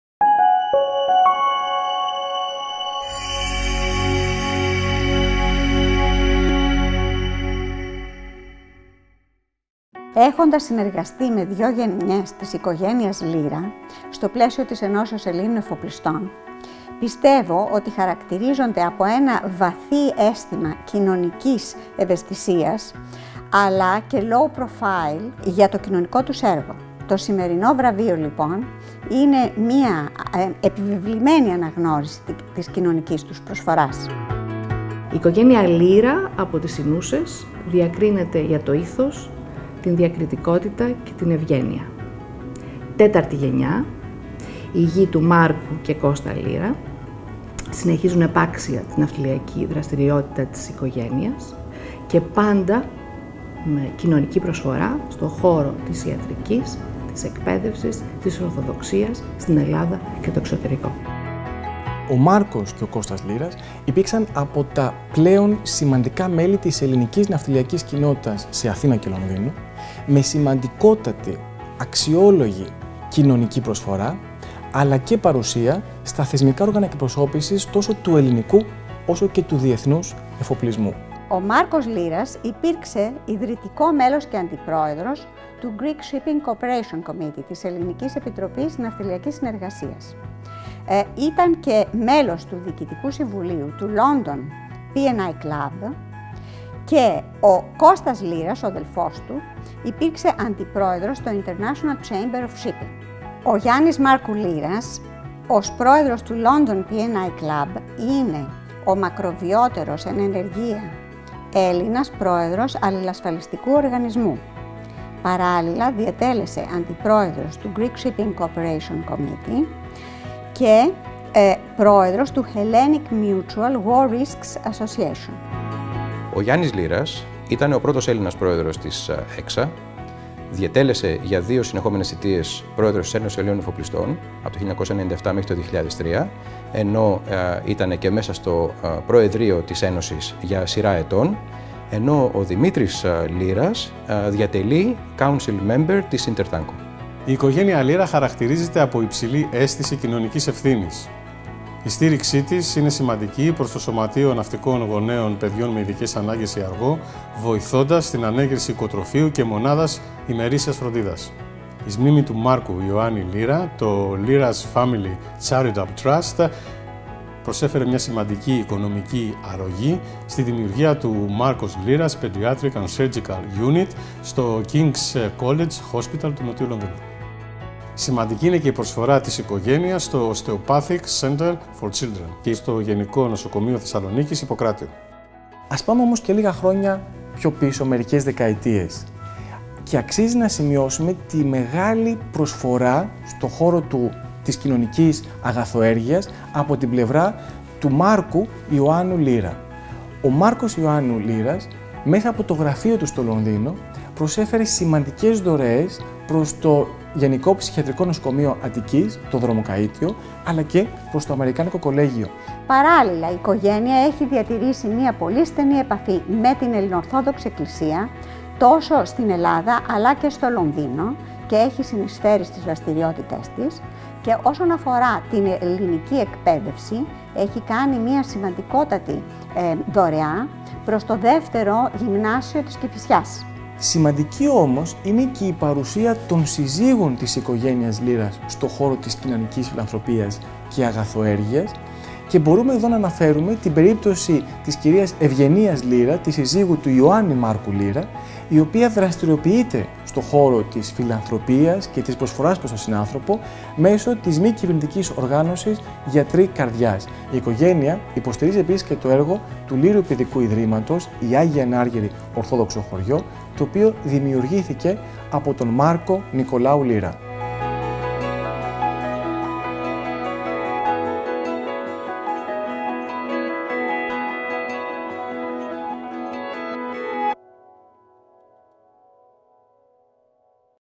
Βραβεία Ευκράντη 2017- Οικογένεια Λύρα (Απόγονοι Ιωάννου Μάρκου Λύρα, 1878-1926)
Το βραβείο κοινωνικής προσφοράς από ναυτιλιακή προσωπικότητα ή φορέα απονεμήθηκε στην Οικογένεια Λύρα (Απόγονοι Ιωάννου Μάρκου Λύρα, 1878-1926), για τη μακροχρόνια κοινωνική δράση της Οικογένειας.

Τα βραβεία «Ευκράντη» που διοργανώνουν κάθε χρόνο τα Ναυτικά Χρονικά, με την αρωγή και τη στήριξη του Ιδρύματος Αικ. Λασκαρίδη, απονεμήθηκαν στις 9 Μαϊου 2018, στην Ιστορική Βιβλιοθήκη του Ιδρύματος Αικατερίνης Λασκαρίδη στον Πειραιά.